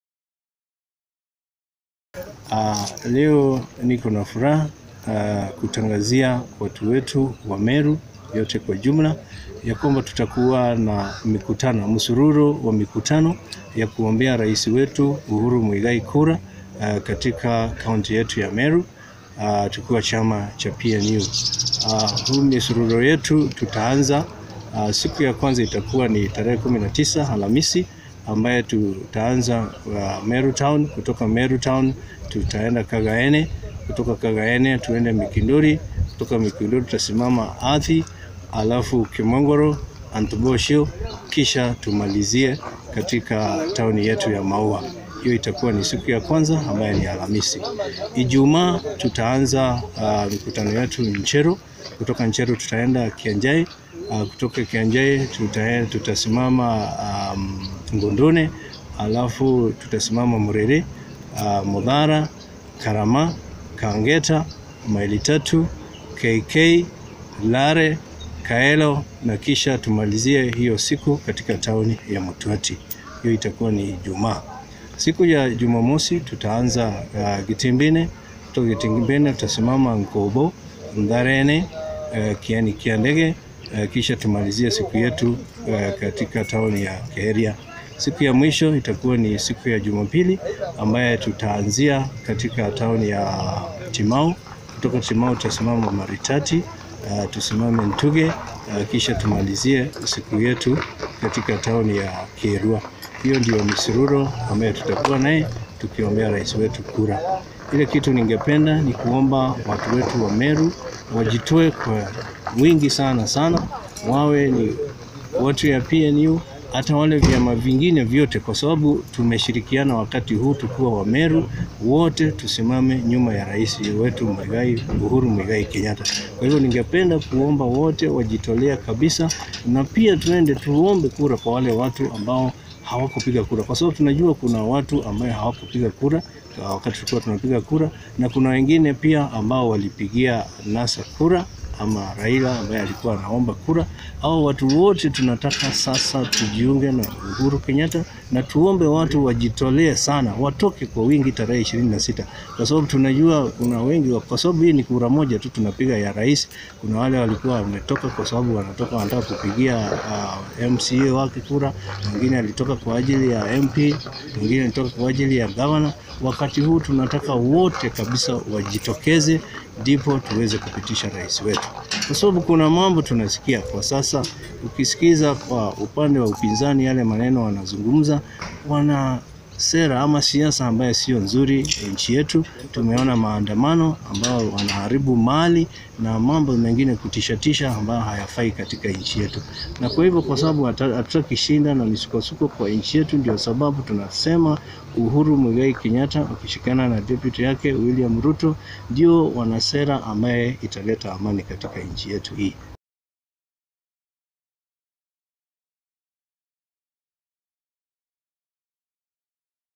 PNU brigade to campaign for UhuRuto in Meru
PNU party national treasurer Peter Kaberia, also former Meru governor Peter Munya's running mate, says from Thursday to Sunday the PNU brigade will campaign in Meru county for UhuRuto.